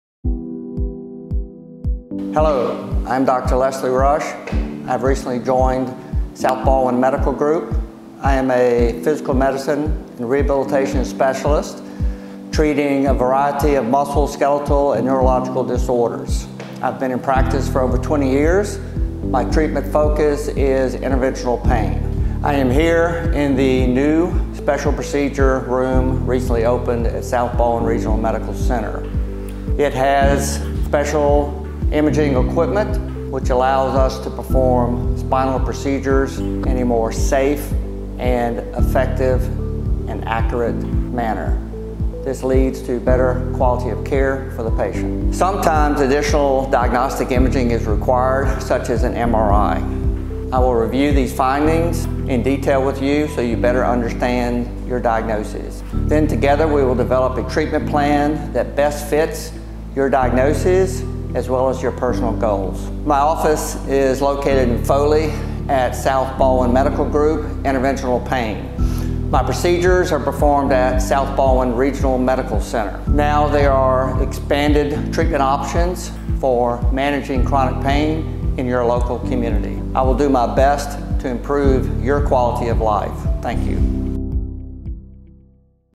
Leslie Rush, D.O. - South Baldwin Medical Group Interventional Pain
Dr. Leslie Rush has joined South Baldwin Medical Group Interventional Pain in Foley and is now accepting new patients!